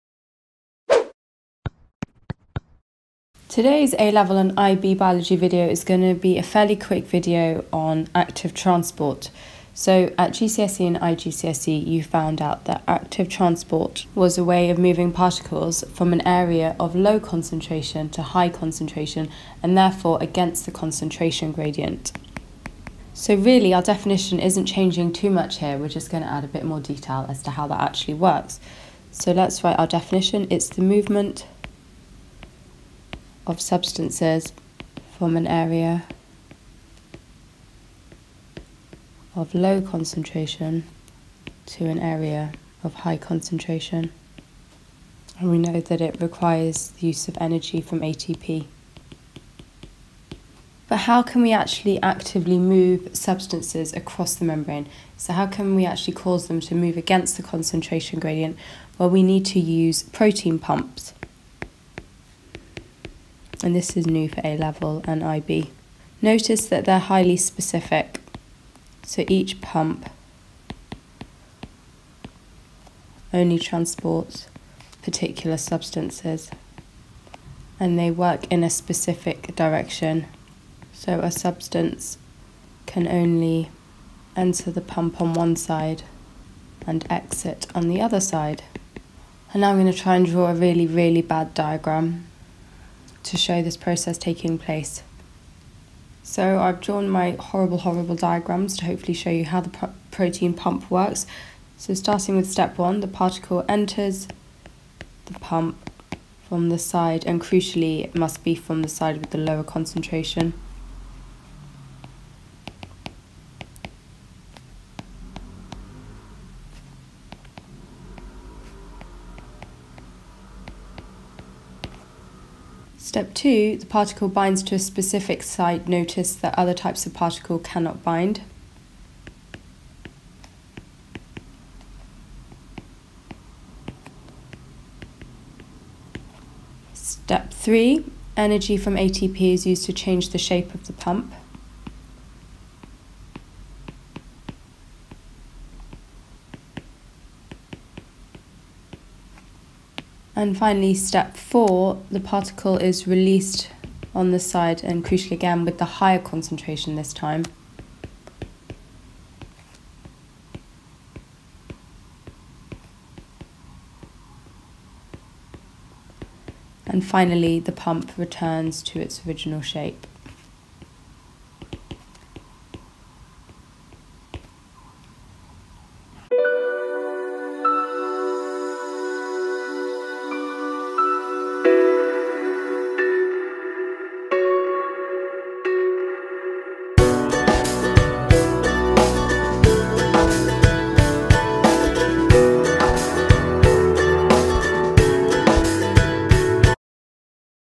Active Transport | A Level & SL IB Biology
Hazel takes you through the process of active transport, in particular across the cell surface membrane. This includes the role of carrier proteins, and why the process requires energy from ATP.

These videos are designed to help with your A level and SL IB science revision.